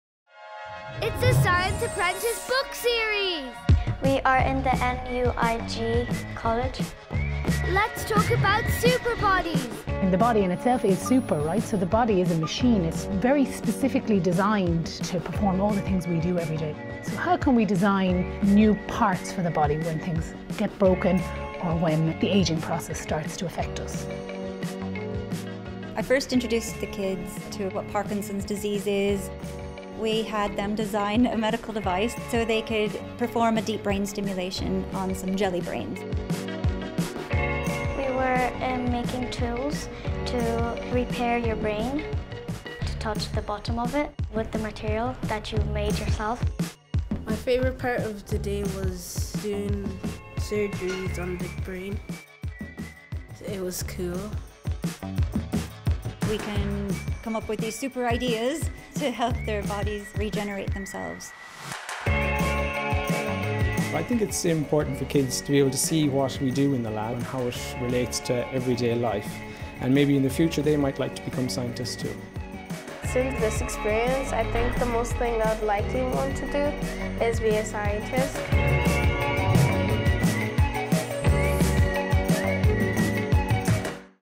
SUPERBODIES - Science Apprentice 2018
Find out how SUPER your body is with our Science Apprentice Stars from Merlin Woods Primary School in Galway!

We take a trip to the biomedical research labs at CÚRAM in National University of Ireland, Galway to learn about medical devices and how researchers in Ireland are working to improve and help our bodies.

Find out more about Science Apprentice at ucd.ie/scienceapprentice